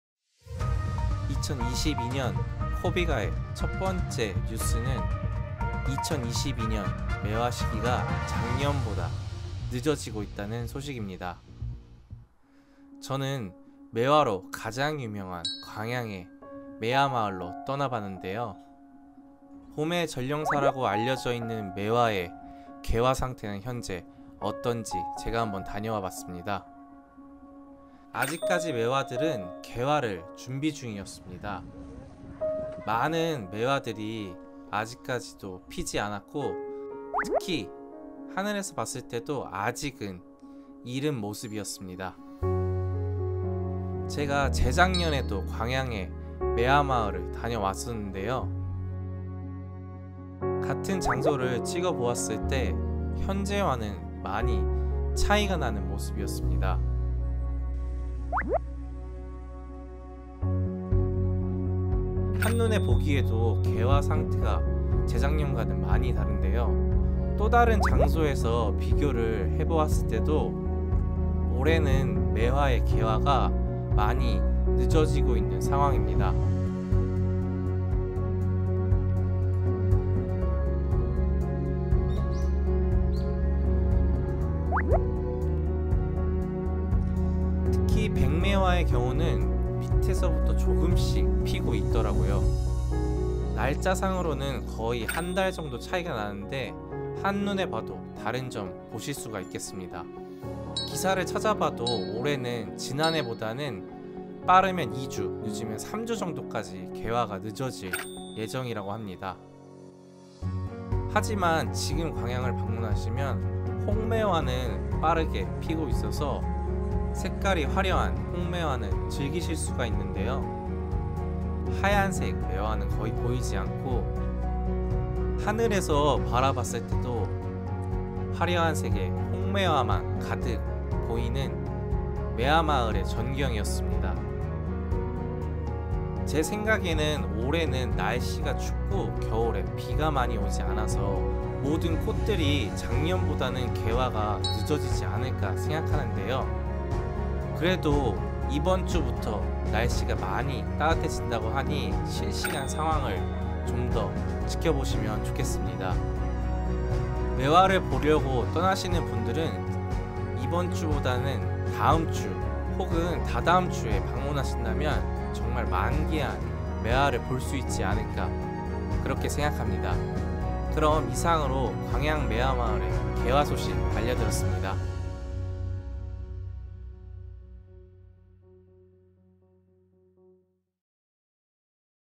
2022 광양 매화 개화 소식! 작년보다 개화시기 늦어져
2022 광양 매화 개화 소식! 작년보다 개화시기 늦어져

코비가 2022년 첫 소식
매화로 가장 유명한 광양 매화마을에 다녀왔습니다
과연 현재 매화 개화 상태는 어떤지 궁금하실텐데요

올해는 작년보다는
개화가 늦어질 예정입니다
제가 방문했을 때도 홍매화만 개화하고 백매화는 아직 많이 기다려야
될 것 같습니다